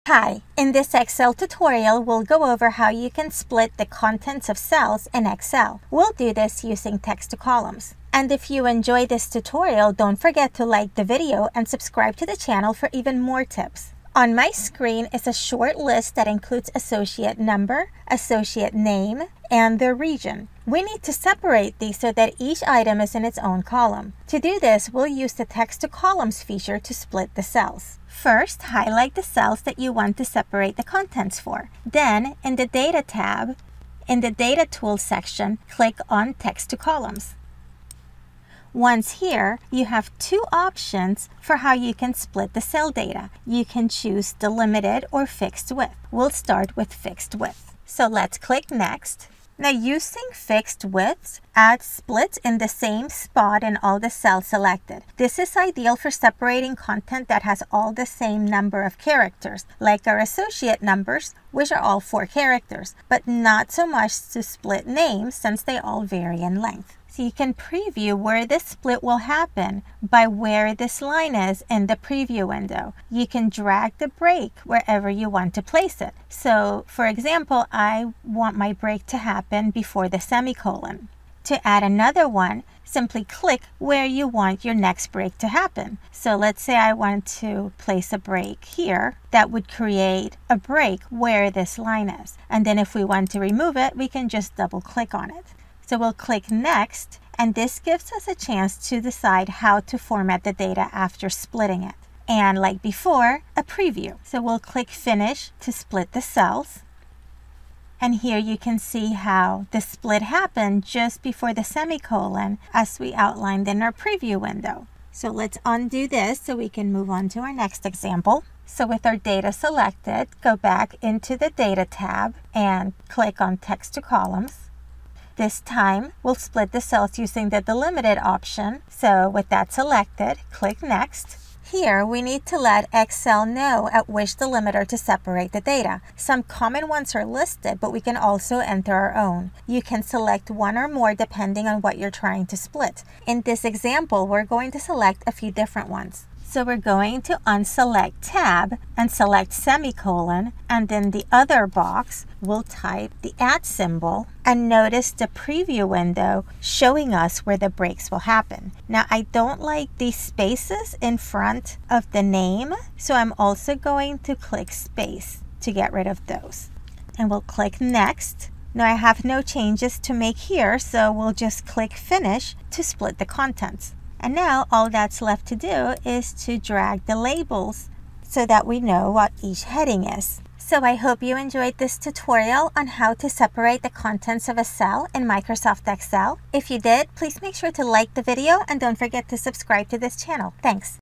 How to Split Cells in Excel with Text to Columns - Excel Tutorial
Excel tutorial on how to split the contents of cells in Excel using Text to Columns.

Using Text to Columns, you have 2 options for how you can split the cell data. You can choose Delimited or Fixed Width.

Fixed Width adds splits in the same spot in all the cells selected and is ideal for separating content that has all the same number of characters.

When you use Delimited to separate cells, you can select 1 or more delimiters (spaces, comma, semicolon, etc) for which to separate your cells at. This is ideal for separating content that varies in length, like names, and in which all the cells in your selection share a common delimiter.

And make sure to get your FREE pdf list of top Excel keyboard shortcuts

Thanks!